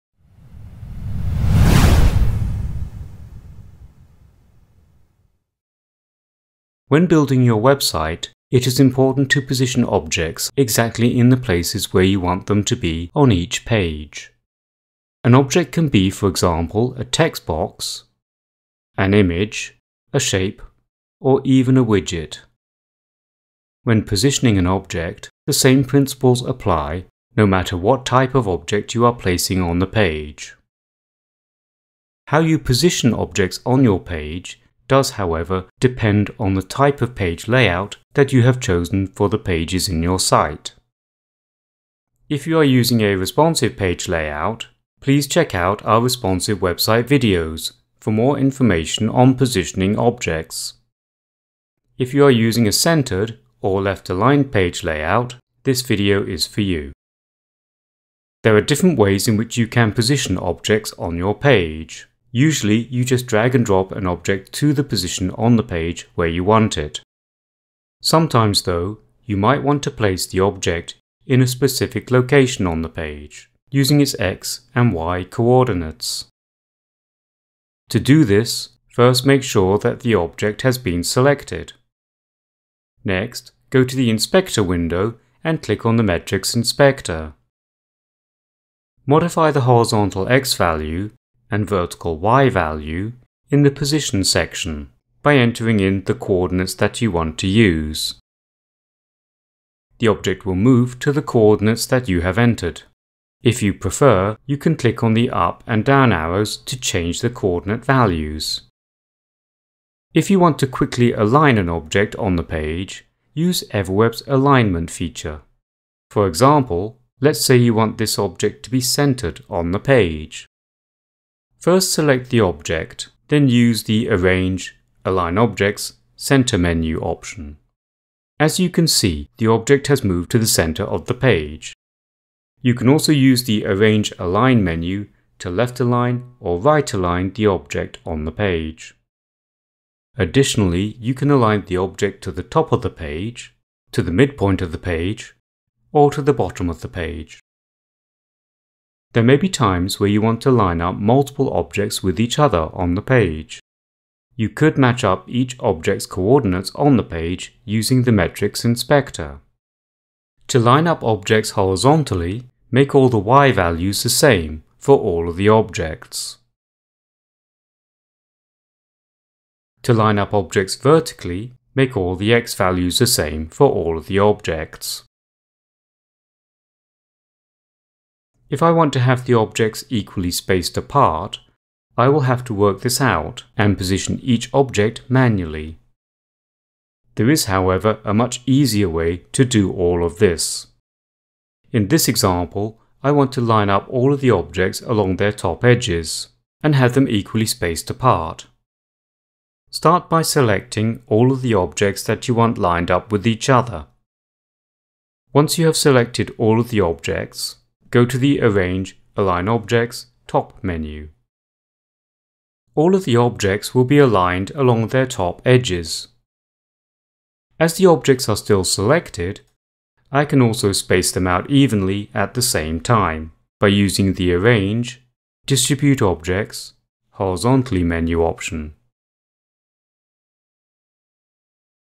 Positioning Objects in EverWeb's Editor Window
EverWeb: The easiest drag and drop website builder EVER!

When building your website, it is important to position objects, exactly in the places where you want them to be on each page.

An object can be, for example, a TextBox, an image, a shape or even a widget. When positioning an object, the same principles apply no matter what type of object you are placing.

How you position objects on your pages depends on the type of page layout that you have chosen.

If you are using a Responsive Page Layout, please check out our Responsive website videos for more information on positioning objects.

If you are using a Centered or left aligned Page Layout this video is for you.

There are different ways in which you can position objects on your page.  Usually you just drag and drop an object to the position on the page where you want it.

Sometimes, though, you might want to place the object in a specific location on the page using its X and Y co-ordinates.

To do this, first make sure that the object has been selected.Next go to the Inspector Window and click on the Metrics Inspector. Modify the Horizontal X value and Vertical Y value in the Position section by entering in the coordinates that you want to use.

The object will move to the coordinates that you have entered.

If you prefer you can click on the up and down arrows to change the coordinate values.

If you want to quickly align an object on the page, use EverWeb’s alignment feature. For example, let’s say you want this object to be centered on the page. First select the object then use the Arrange, Align Objects, Center menu option.

As you can see the object has moved to the center of the page.

You can also use the Arrange, Align menu to left align, or right align the object. In addition, you can align the object to the top of the page, to the mid point of the page or to the bottom of the page.

There may be times where you want to line up multiple objects with each other on the page. You could match up each object’s coordinates on the page using the Metrics Inspector.

To line up objects horizontally, make all the Y values the same for all of the objects.

To line up objects vertically, make all of the X values the same for all of the objects.

If I want to have the objects equally spaced apart I will have to work this out and position each object manually. There is, however, a much easier way to do all of this...

In this example, I want to line all of the objects along their top edges
and have them equally spaced apart.

Start by selecting all of the objects that you want lined up with each other.

Once you have selected all of the objects, go to the Arrange, Align Objects, Top menu. All of the objects will be  aligned along their top edges.

As the objects are still selected, I can also space them evenly at the same time by using the Arrange , Distribute Objects , Horizontally menu option.